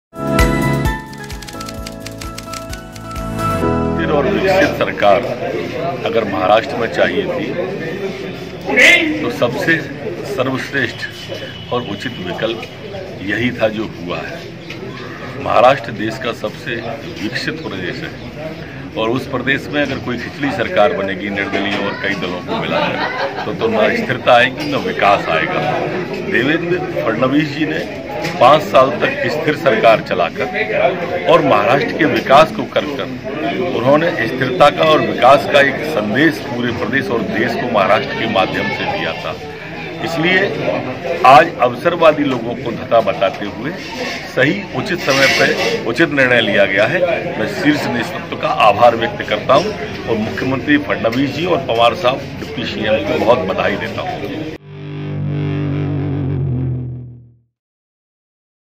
महाराष्ट्र में उलटफेर के बीच पूर्व मंत्री और भाजपा विधायक नरोत्तम मिश्रा ने दिया ये बयान
महाराष्ट्र में उलटफेर के बीच पूर्व मंत्री और भाजपा विधायक नरोत्तम मिश्रा ने दिया ये बयान#Narottam Mishra# Mumbai#Devendra Fadnavis took oath as Maharashtra# Chief Minister NCP# Ajit Pawar Deputy CM of the state. Raj Bhawan